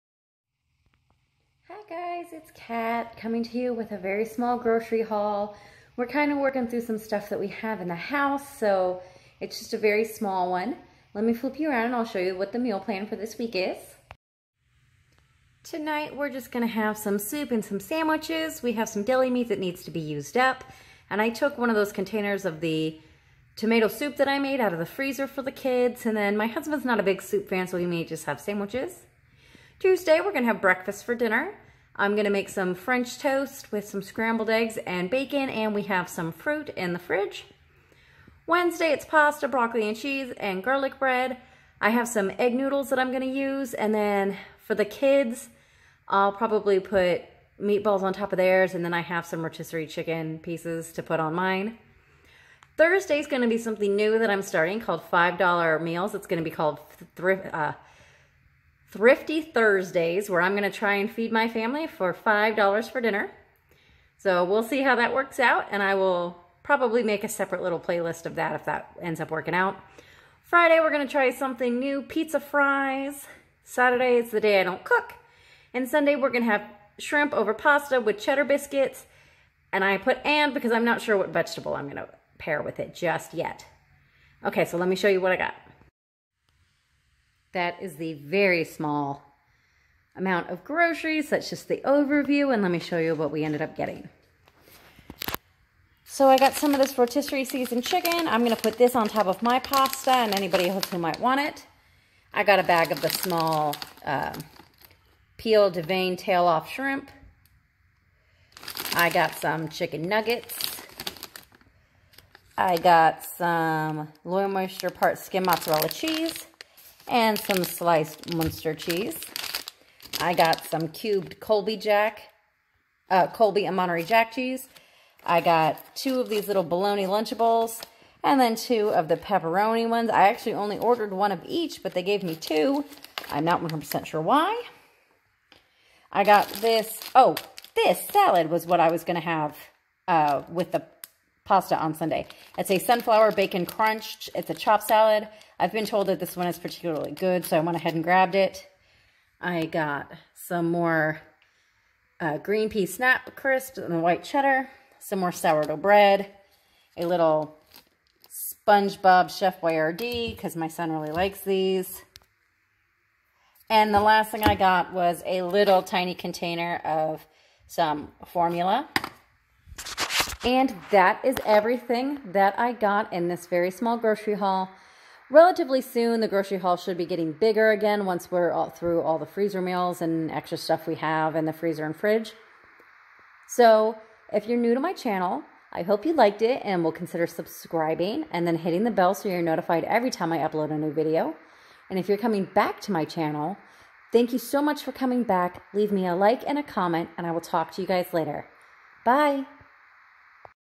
Grocery haul
Here's just a small grocery haul for this week. I apologize for the hot mess state I'm in, it was a long night and it's been a rough day so far.